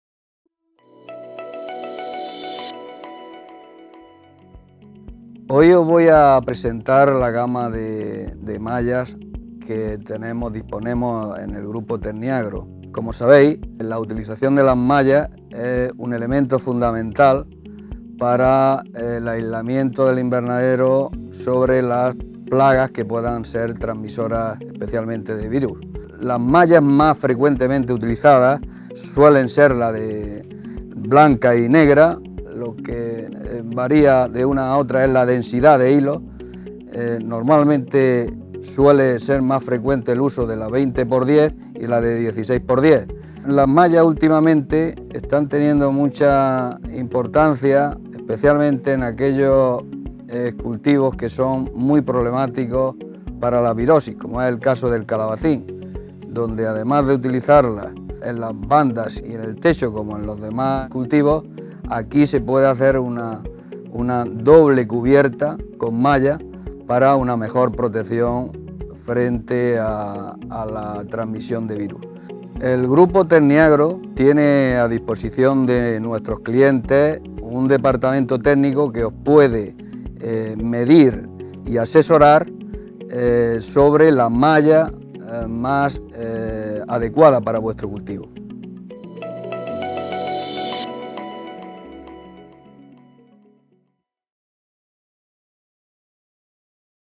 Soluciones Agrotextiles Grupo Tecniagro
Desde Grupo Tecniagro suministramos Agrotextiles a todos nuestros agricultores y colaboradores. Nuestro valor añadido a la hora de comercializar mallas y otros plásticos agrícolas, es la de realizar mediciones precisas, asesoramiento técnico y personalización para cada cliente.